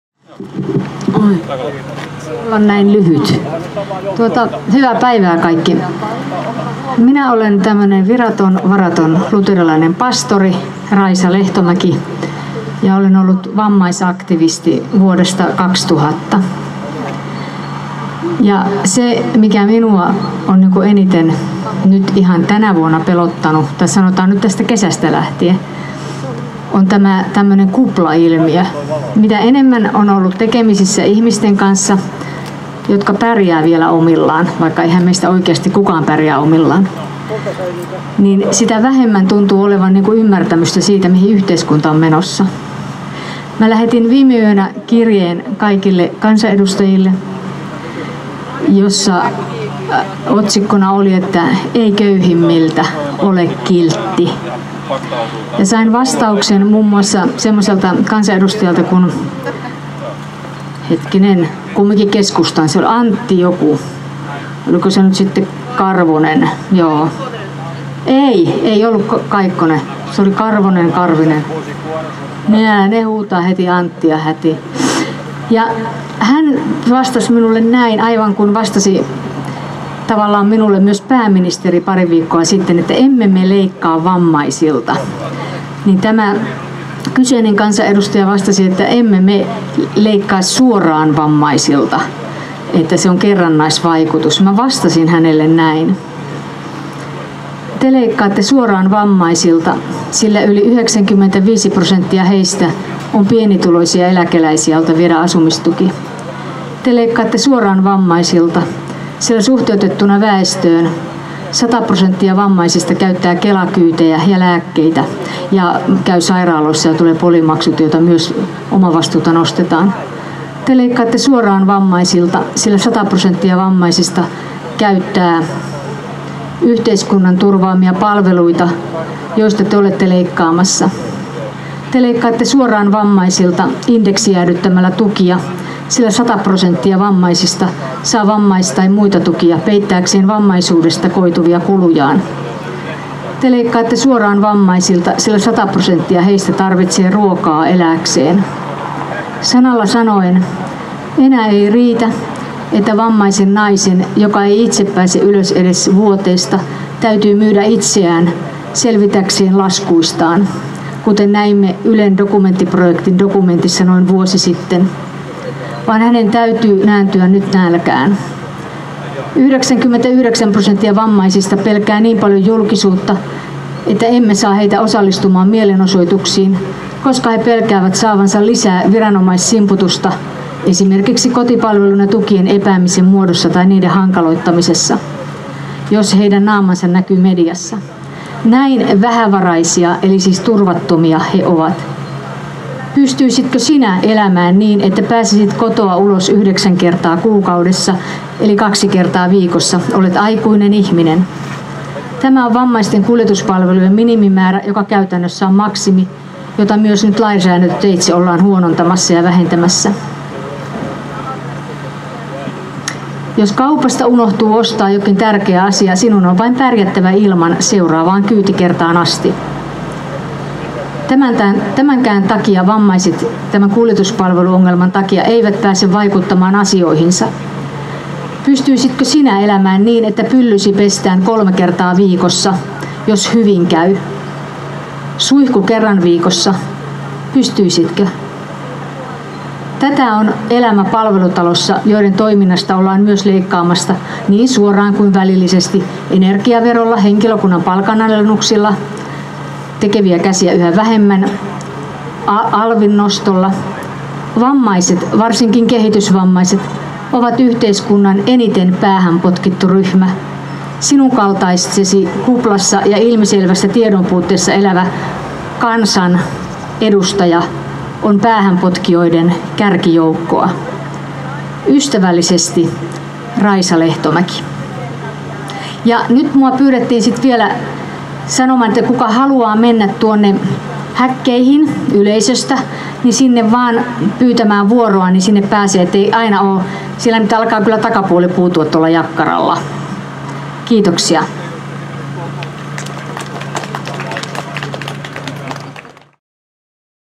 Hallitus leikkaa suoraan vammaisilta
Omaishoitaja ja pappi Raisa Lehtomäki puhuu Joukkovoiman Ihmistarhaus-mielenosoituksessa budjettiriihen yhteydessä Säätytalolla Helsingissä 9.9.2015.